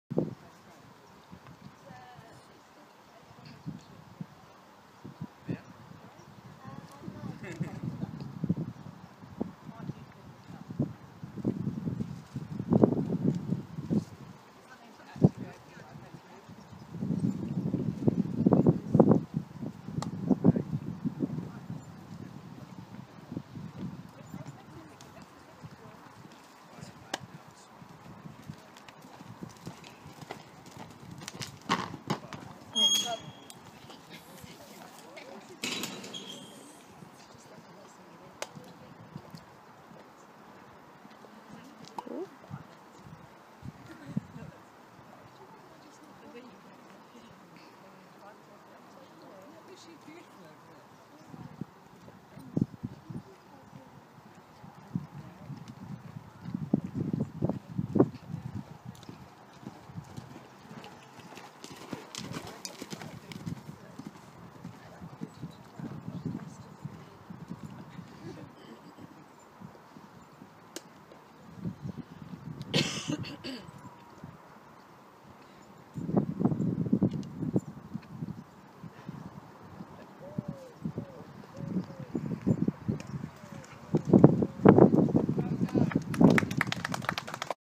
Dolly - Clear Round 17th July
She was being a lil bit lazy but I am still really happy with her for jumping everything :-) One unlucky pole.